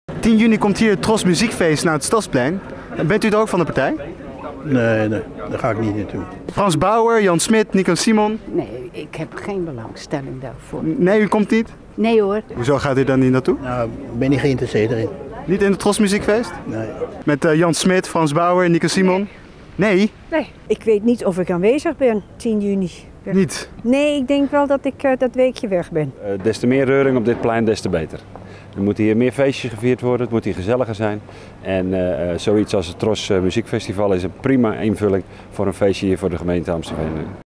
Gaat u naar het TROS Muziekfeest op het Plein? - RTVA
Over twee weken is het Amstelveense Stadsplein het epicentrum van Nederlandse muziek. Alle toppers die het Nederlandse lied zingen zullen aanwezig zijn bij het TROS Muziekfeeest op het plein. Maar houdt de Amstelvener wel van de 3J's, Gerard Joling of Wesley Bronckhorst?